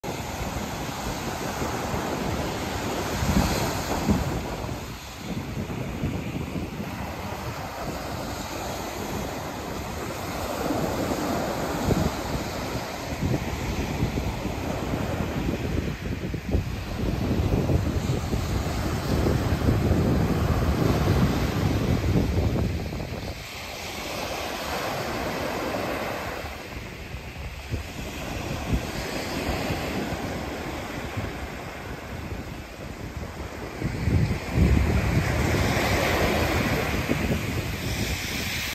七里御浜海岸 熊野市
20キロ続いてる海岸で
海の色は蒼くて白い波が
綺麗だった💕